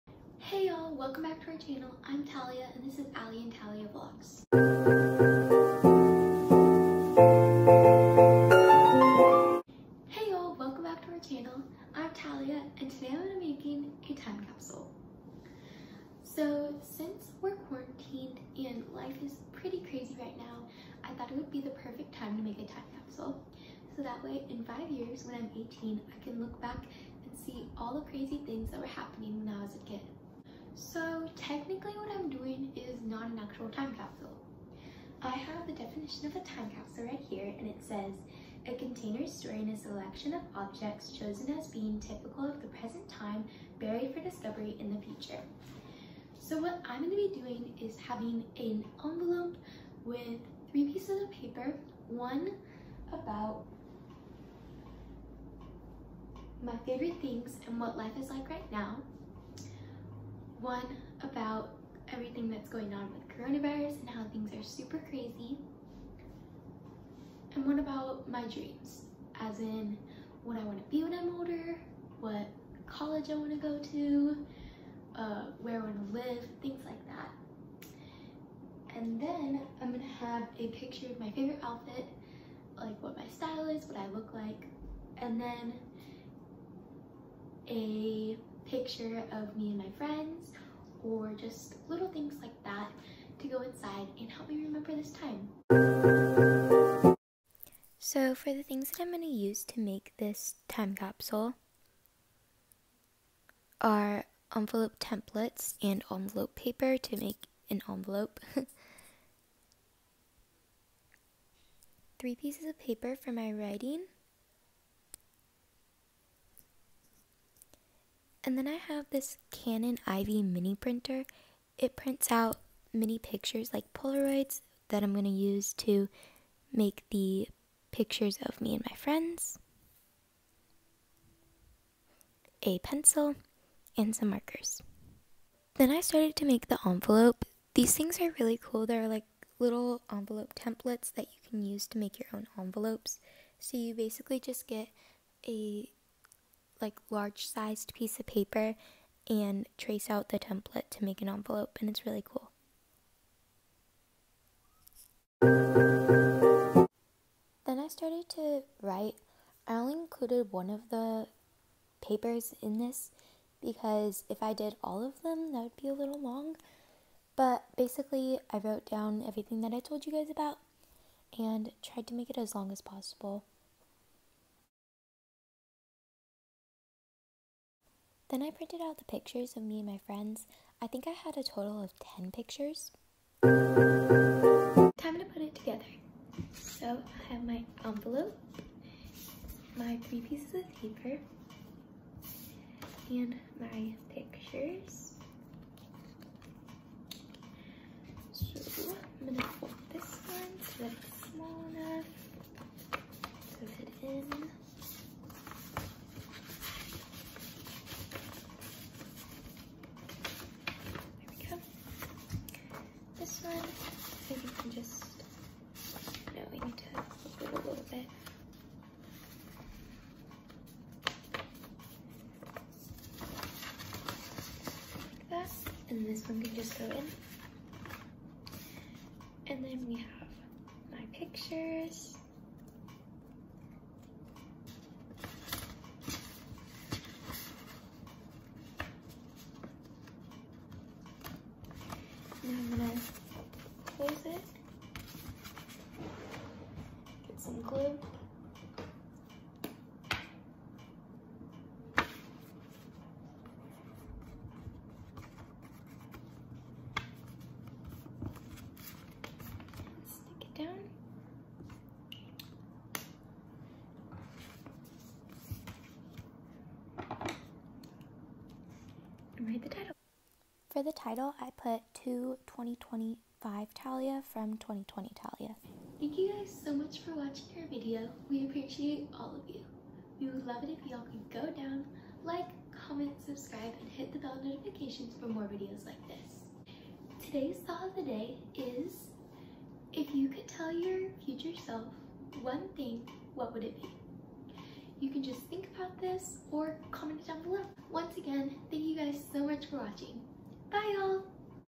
Making A Time Capsule
Today i’m gonna be making a envelope including 3 pieces of paper that express, what life is like right now, corona craziness, and one about my dreams which included what i want to be when i’m older, what collage i want to go to, where i want to live and other fun things like that.  Also i included about 10 photos of me and my friends.  Today’s thought of the day is, if you could tell your future self one thing what would it be?  Bye Y’all ❤️